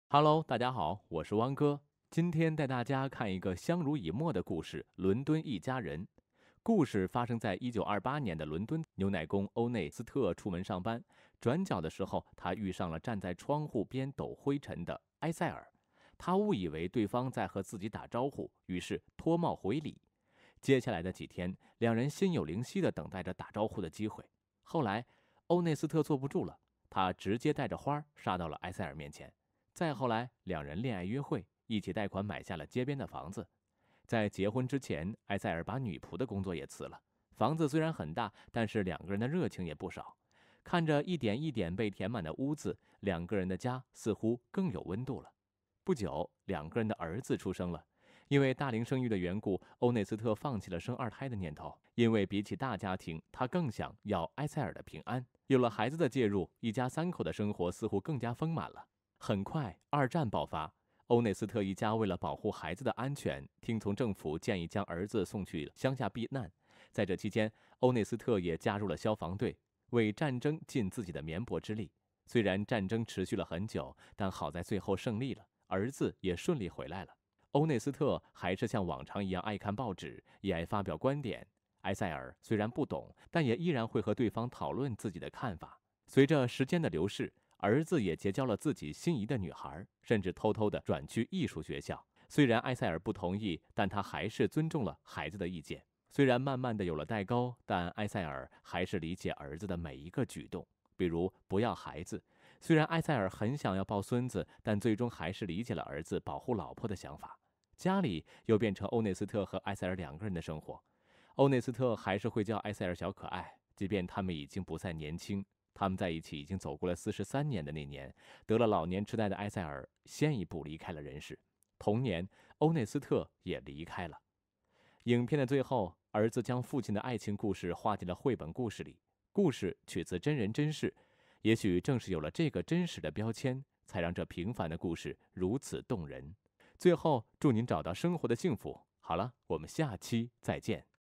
爱就是一起慢慢变老，他的父亲母亲，活成了我们最想要的样子
电影、影视、动画、家庭、爱情